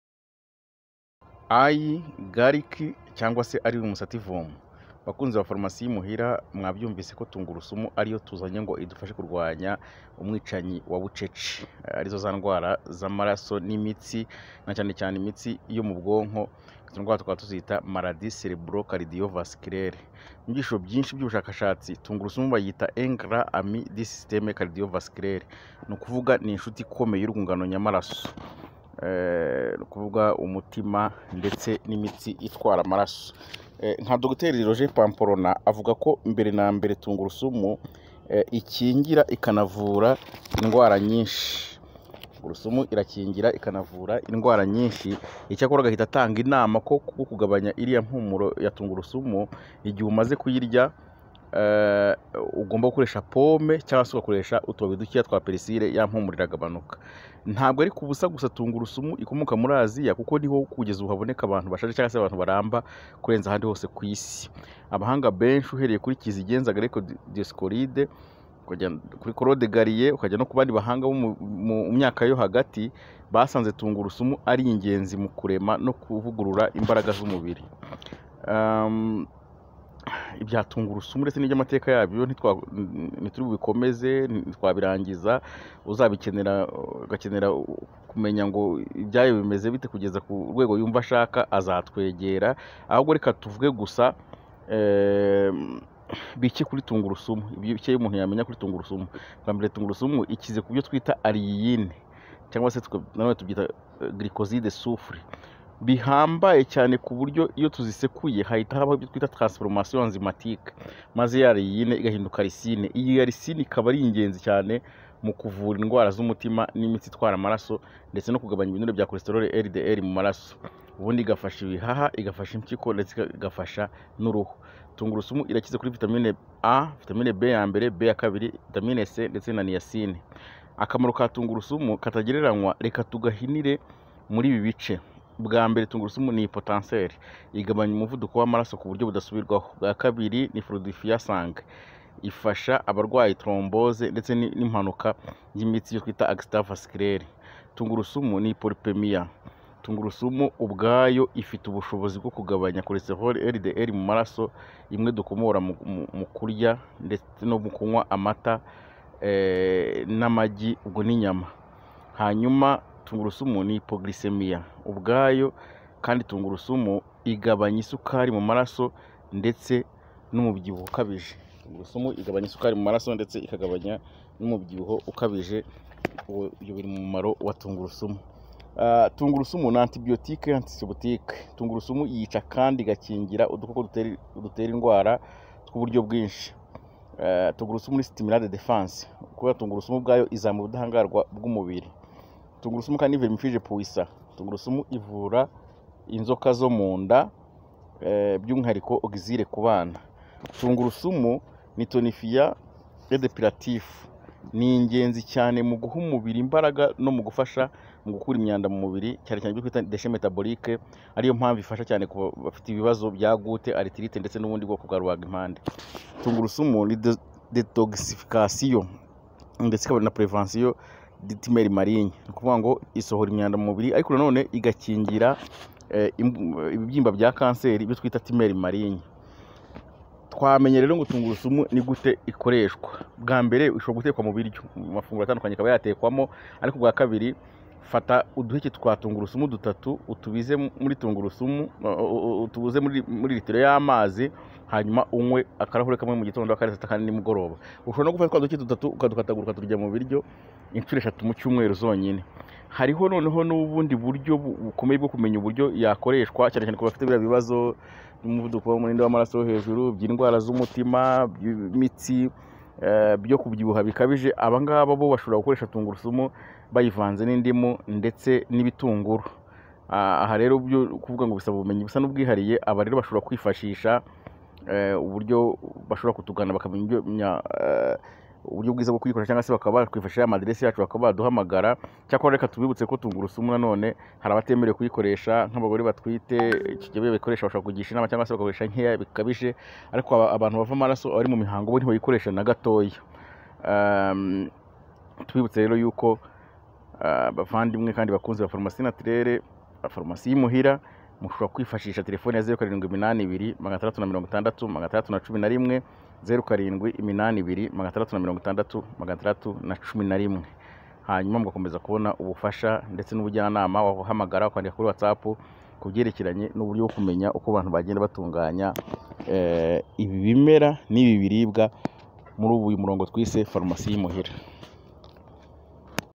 FARUMASI Y'I MUHIRA(Igice cya 2): TUNGURUSUMU
Menya ibyiza bya Tungurusumu,Impamvu abo kumugabane w'Aziya baramba cyane , Tungurusumu ibifitemo uruhare runonosoye